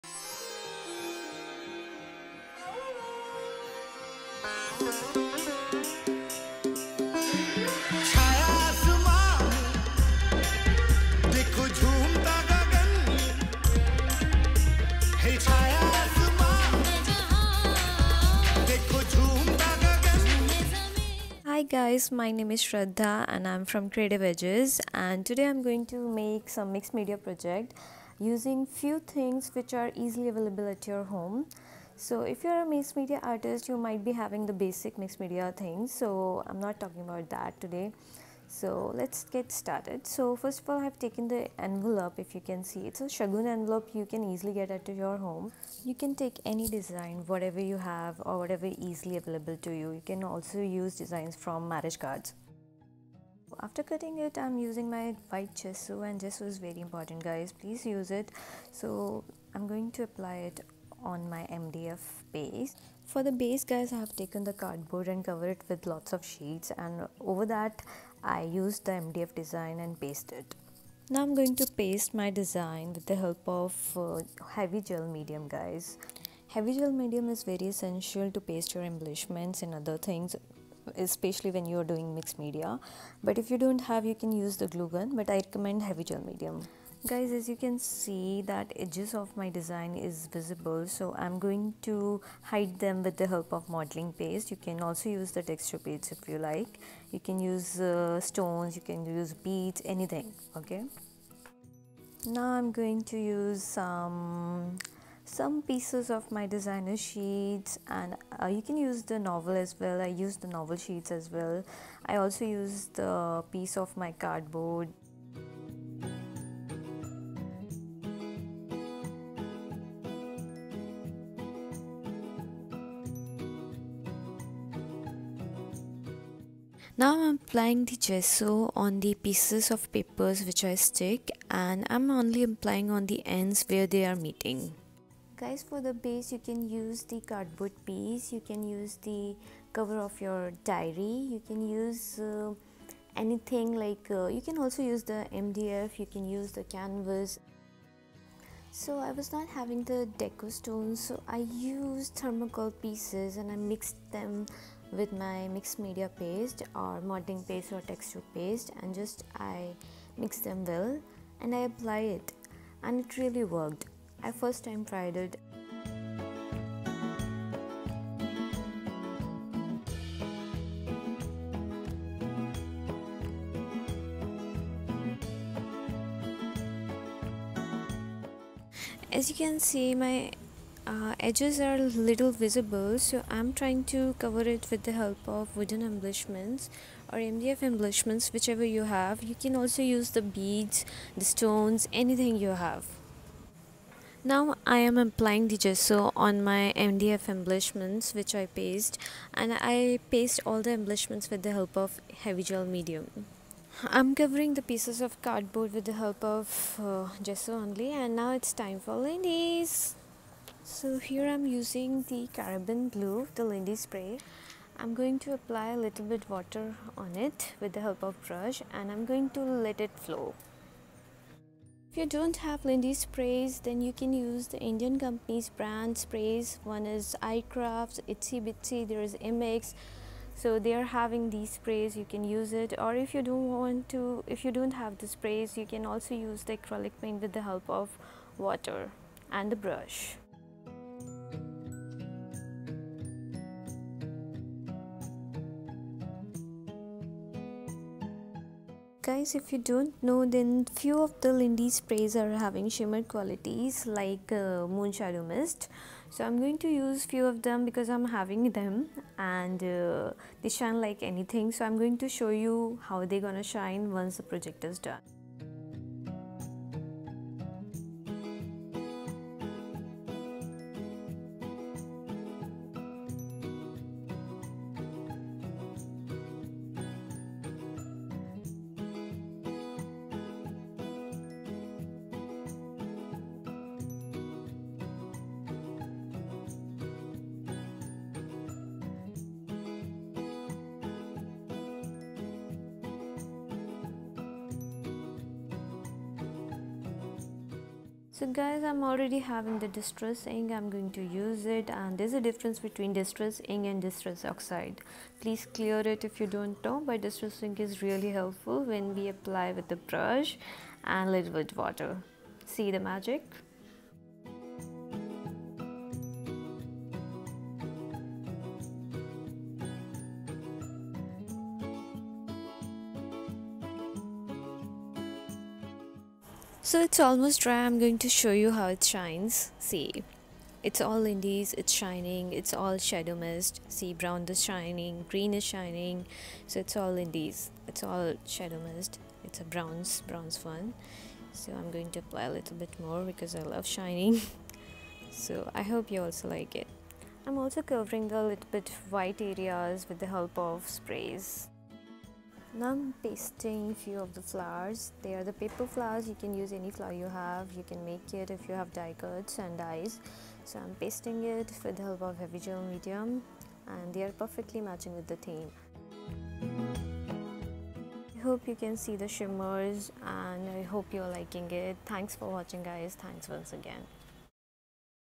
FREE MIXEDMEDIA TUTORIAL BY CREATIF EDGES
This is free and easy very easy mixed media tutorial , if you have any doubts and queries please what’s app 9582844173
The products I used :-
Lindys
- flat Fabio - Caribbean blue
- star burst - sea mint green
- moon shadow mist - mystic malachite
- moon shodow mist - buccaneer bronze
Others
-gesso - craft junction
-mixed media paste - little birdie
- stencil - itsy bitsy
- heavy gel medium -little birdie